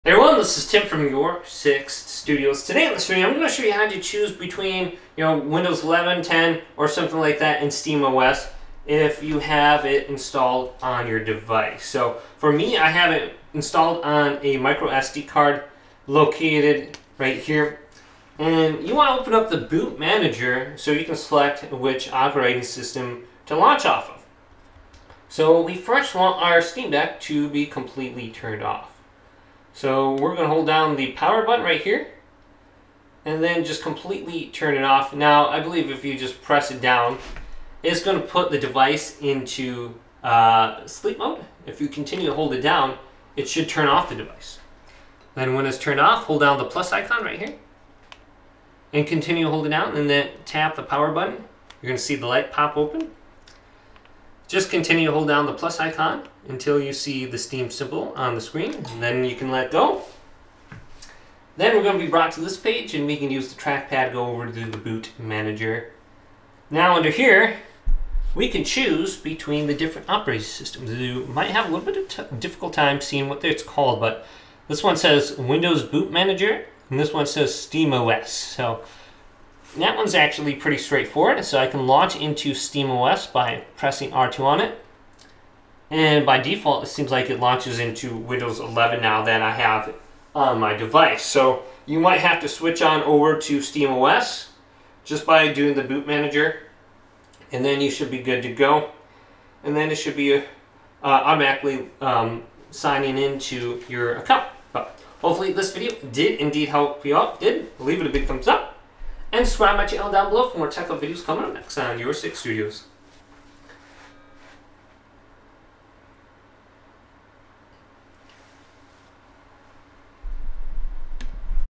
How to Choose Operating System with Boot Selection on Steam Deck (Windows or Steam OS)
To choose the operating system on your Steam Deck using the boot selection menu, follow these steps:

Power Off: Completely power off your Steam Deck.
Enter Boot Manager: Hold the Volume Down button and press the Power button. Release both buttons once you hear a sound.

Select OS: You will be taken to the boot selection menu. Use the joystick or D-pad to select the operating system you want to boot into (e.g., SteamOS or Windows) and press the A button to confirm